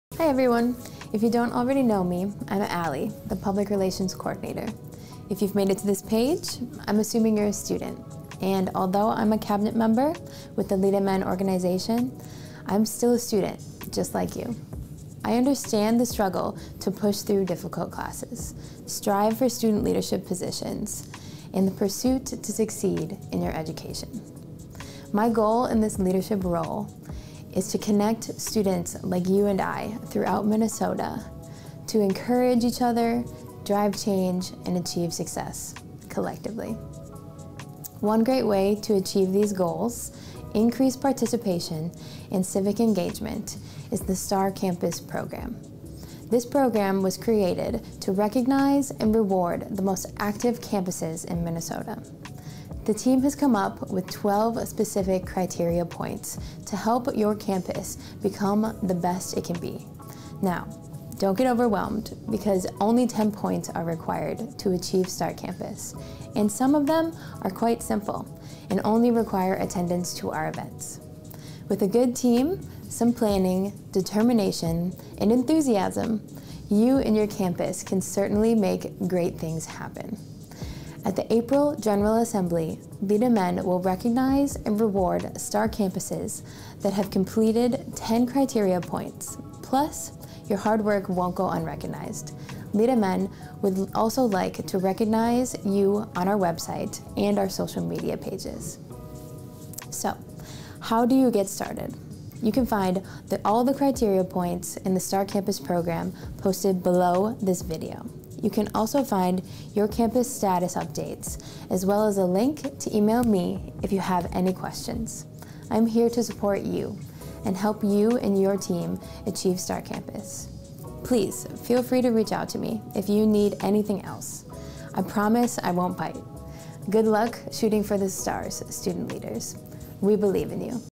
Become a Star Campus
Listen as PRC Ali Tomashek describes how to get involved with LeadMN's Star Campus Program.